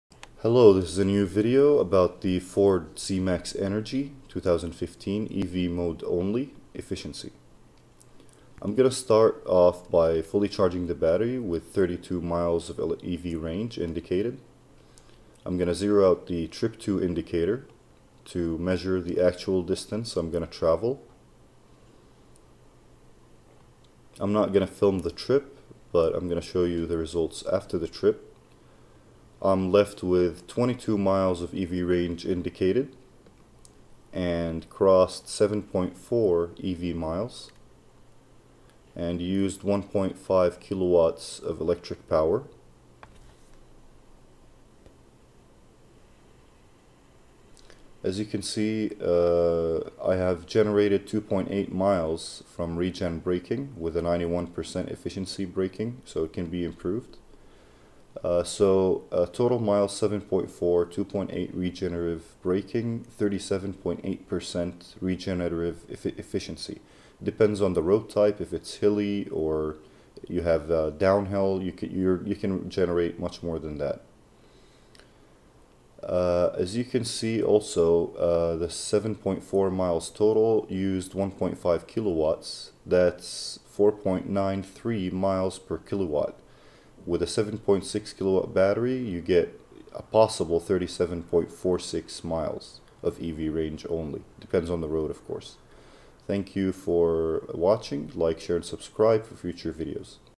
Ford C max Energi EV Mode only Range Efficiency
Ford C max Energi EV only Mode Range Efficiency. this video estimates the possible Ford C max Energi EV only range by using a way to calculate Electric only range efficiency موقع لقطع السيارات